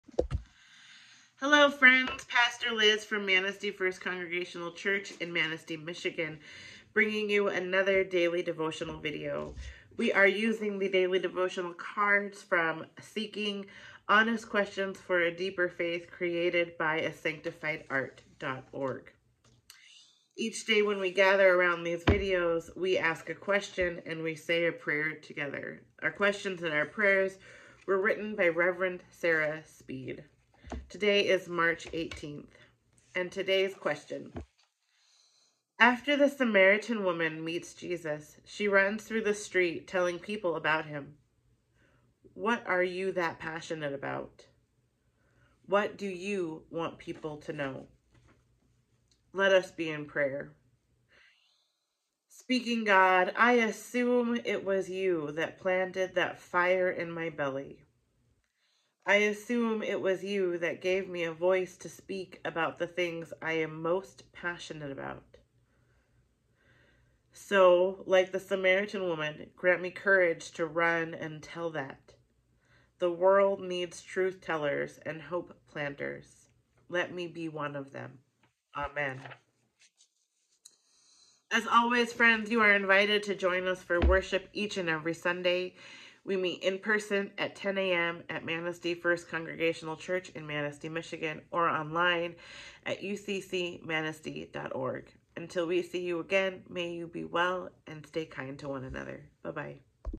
Daily Lenten devotional videos 3 18 2023
Prayers by Rev. Sarah Speed at Sanctified Art.org